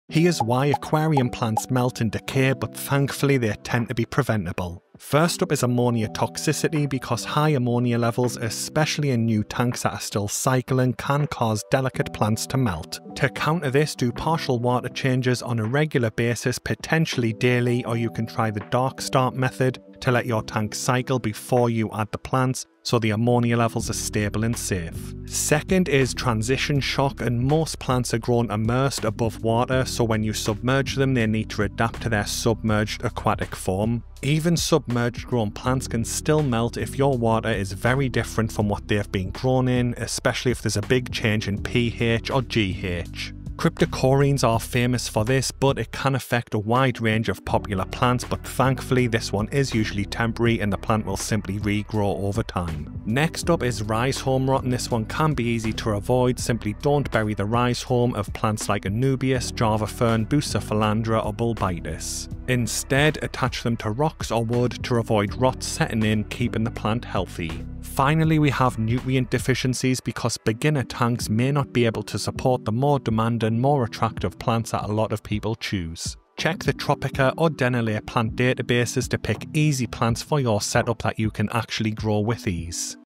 Why Aquarium Plants Melt! - Stop Aquatic Plant Melt In Your Planted Tank!
Wondering why your aquarium plants are melting, turning brown, or just disintegrating after adding them to your fish tank? 😓 You’re not alone—it’s one of the most common struggles in the aquarium hobby. But the good news is: most of the causes are preventable, and many plants bounce back with just a little patience and the right care. If you're setting up a planted tank or maintaining a mature planted aquarium, this info can save your aquatic plants from an early meltdown! 🌿🐠

Here are 4 major reasons your aquarium plants might melt or decay:

1️⃣ Ammonia Toxicity
High ammonia levels, especially in new tanks, can kill off delicate aquatic plants very quickly. During the nitrogen cycle, ammonia spikes are common, and plants aren’t always able to handle that stress.
✅ Tip: Do partial water changes every 2–3 days during cycling to keep levels manageable, or try the Dark Start method—cycling the tank without plants or animals first—so by the time you plant, ammonia is already under control. This gives your planted tank a healthier start.

2️⃣ Transition Shock
Most aquarium plants are grown emersed (above water) at farms to speed up growth. Once submerged in your tank, they need time to adapt to the new conditions. This leads to melting as the plant drops old leaves and grows new ones suited for underwater life.
Even plants already grown submerged can melt if there’s a drastic change in water parameters like pH, GH, or KH between the store and your planted aquarium.
🔁 Don’t panic—plants like crypts are especially known for melting, but they often regrow stronger with time.

3️⃣ Rhizome Rot
Popular plants like Anubias, Bucephalandra, and Java Fern have a rhizome—a thick stem that stores energy.
🚫 If you bury this rhizome in substrate, it can rot and kill the plant.
✅ Instead, tie or glue these aquarium plants to rocks, driftwood, or decorations. As long as the rhizome is exposed to water flow, it’ll stay healthy and thrive in your planted tank.

4️⃣ Nutrient Deficiency
A new planted aquarium might not have enough nutrients to support demanding species. Yellowing, holes in leaves, and poor growth can all be signs of deficiencies in nitrogen, potassium, or iron.
✅ Tip: Use beginner-friendly plants like Java Fern, Anubias, Hornwort, or Amazon Frogbit until your tank stabilizes.
Tools like the Tropica or Dennerle plant databases can help you choose aquatic plants suited to your tank size, lighting, and skill level.

Starting a planted aquarium is one of the most rewarding parts of the fishkeeping hobby—but it comes with a learning curve. The key is to understand what your aquarium plants need and give them a stable environment. Melting doesn’t always mean your plant is dying—it often means it’s adjusting.

So if you're seeing some leaf loss in your new fish tank, don’t give up! With patience, your planted tank can become a lush, thriving ecosystem. 💚🌱